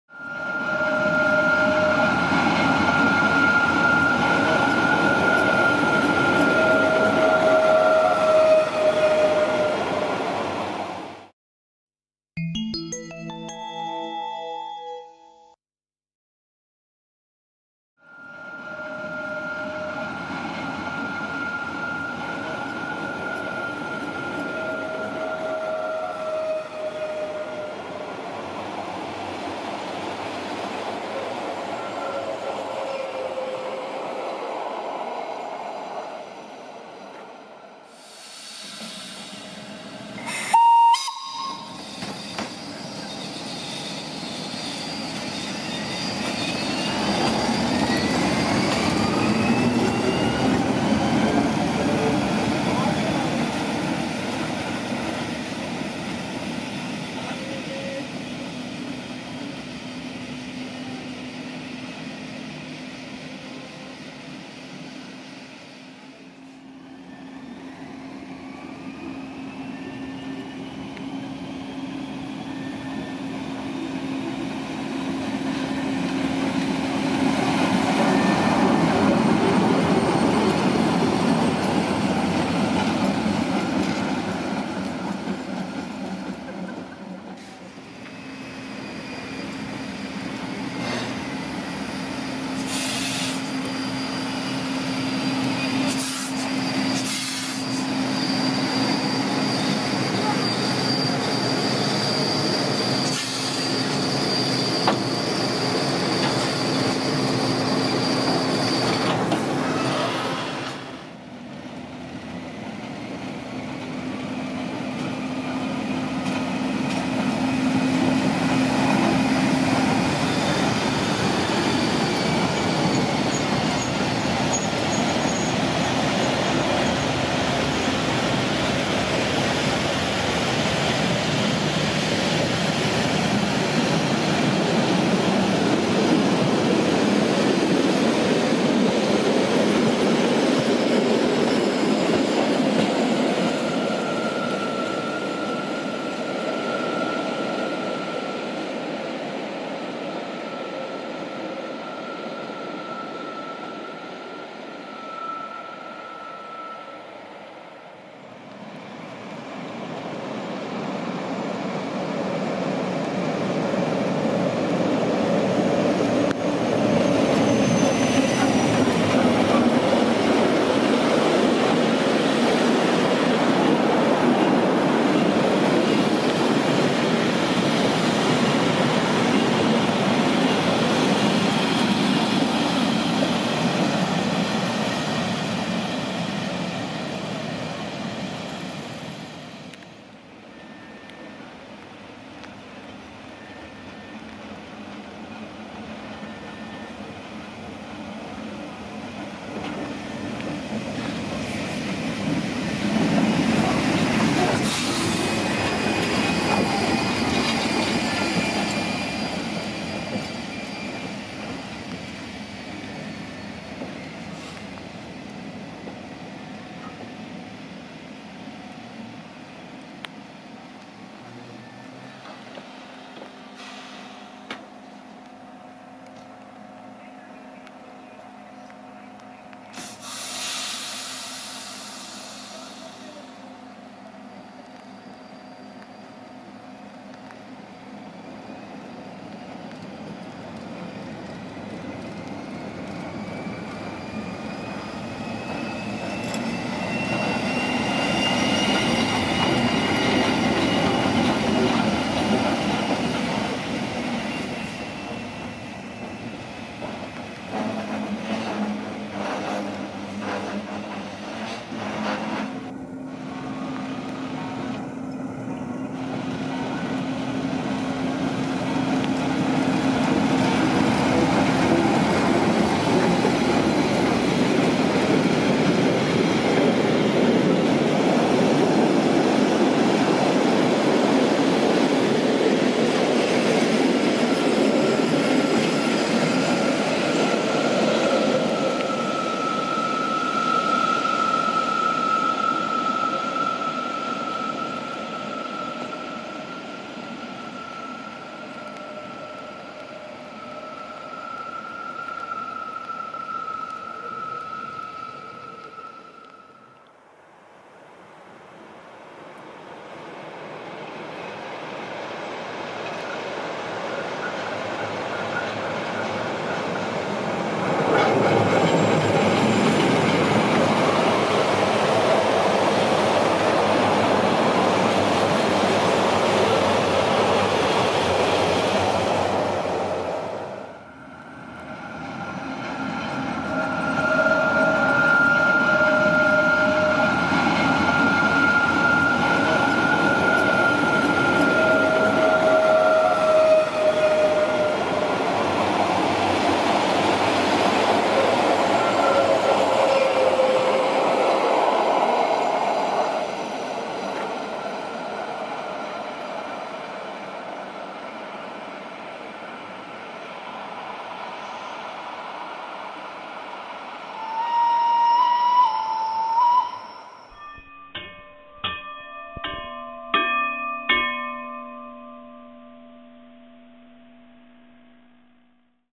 Swiss Trains: Rhätische Bahn / Rhaetian Railways Nostalgia Zug with “Baby Crocodile” at Filisur
Over the Summer months the Rhätische Bahn operated a “Nostalgia Zug” service between Davos Platz and Filisur, twice daily.

These trains ran in lieu of regular services, but with a slightly longer schedule.  No special ticket was required!

Passengers had a choice of riding in historic coaches, including a luxurious first class coach or open air "panorama" wagons.

Historic electric locos were also in charge, most often the Ge 4/4 I class #414, the “Krokodil” or “Baby Crocodile”, built in 1929.

Filmed on 29 & 30 September 2022